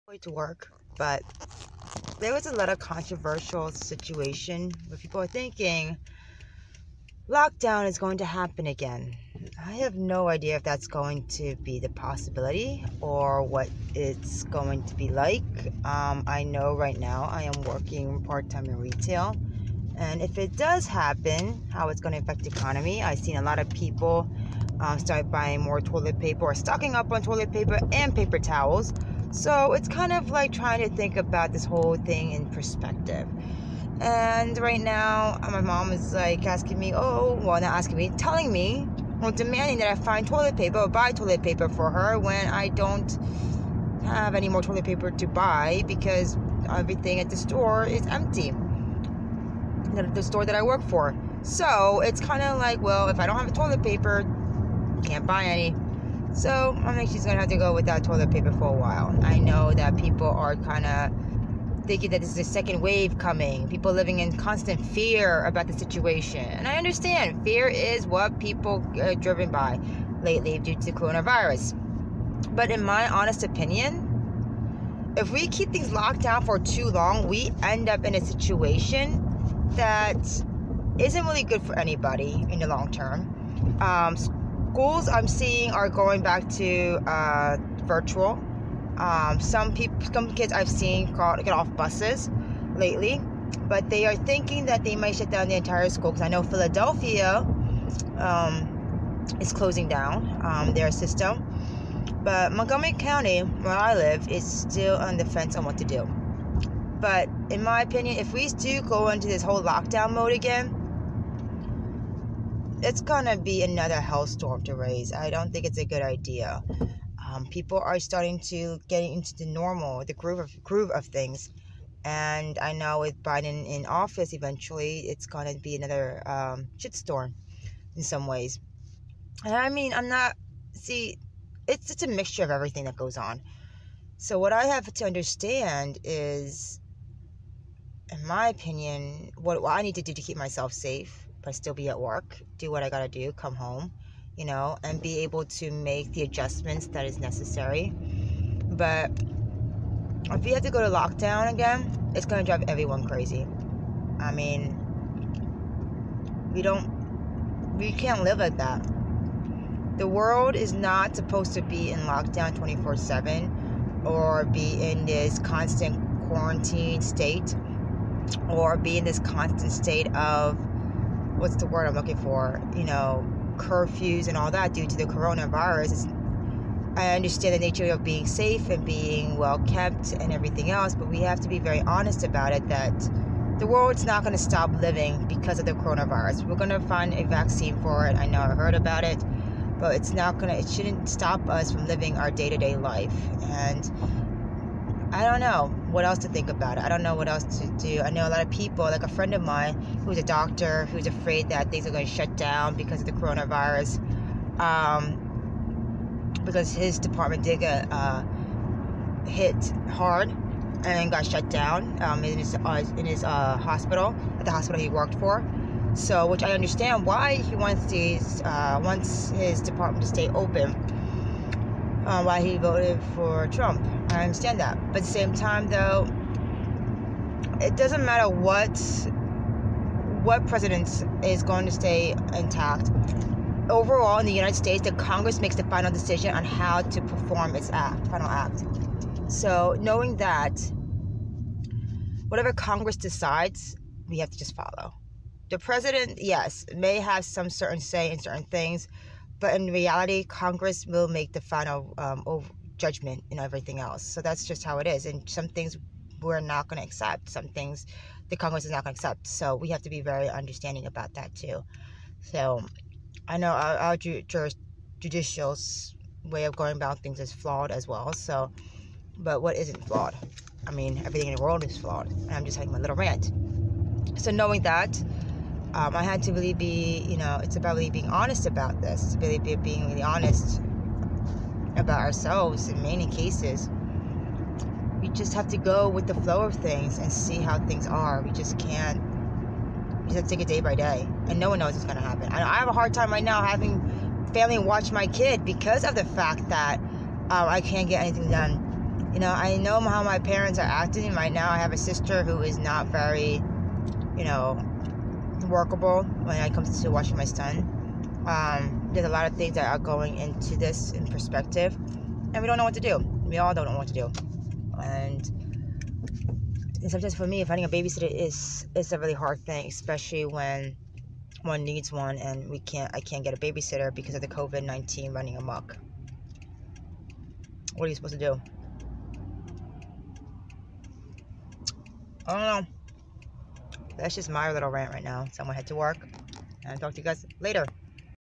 my rant on second lockdown...
Who knows what will actually happen but people are literally are taking this seriously. It's just taking extra precaution that makes all the difference but when does it go too far?

As a working mother myself, I have these thoughts on what to do with child care and finding a babysitter as I work. I am not sure what to do... sometimes with family asking them for help isn't always plausible and a struggle to get because of the covid fear that comes around. It can be very frustrating to deal with. I am right now working part time so money is coming in but not sure how it will last. I am being honest with what I am feeling here. Even though I am healthy here, so is my son sometimes it's like a double edge sword on what to do.